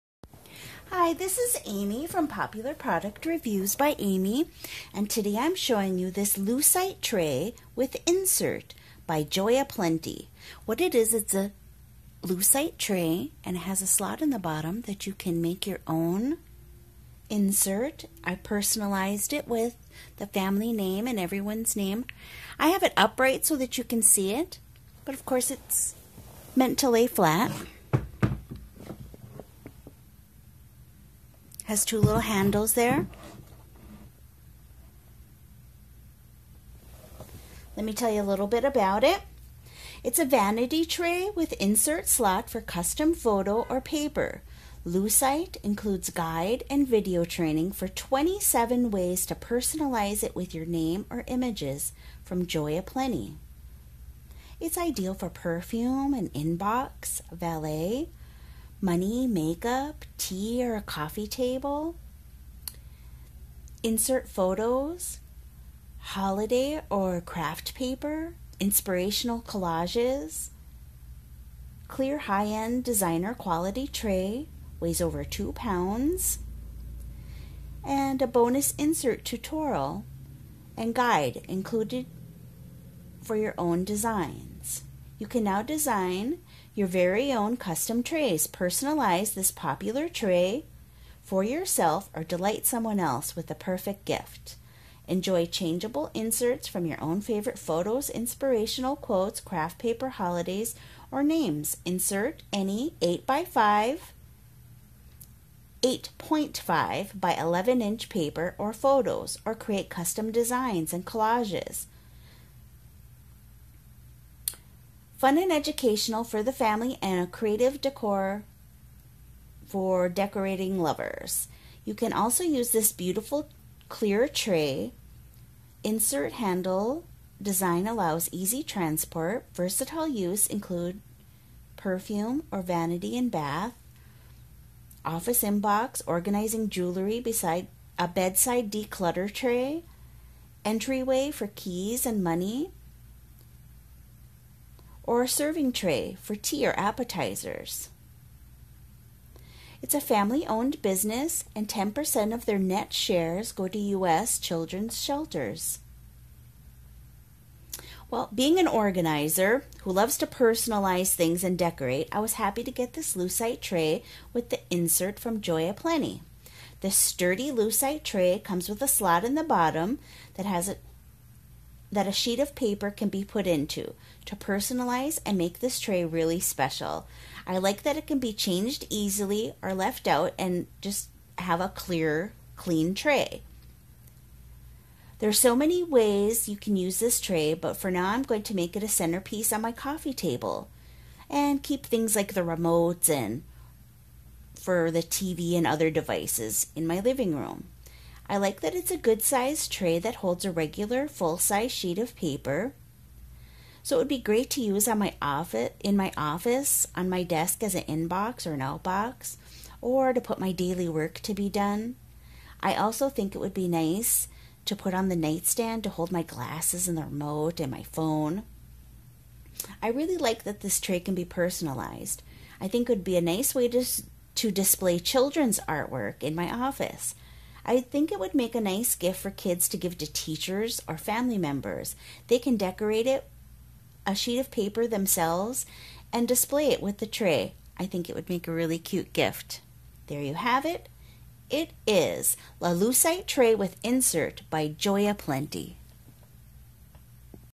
Lucite Tray with Insert by Joyaplenty
Vanity Tray with Insert Slot for Custom Photo or Paper - Lucite - Includes Guide and Video Training for 27 Ways to Personalize It with Your Name or Images from Joyaplenty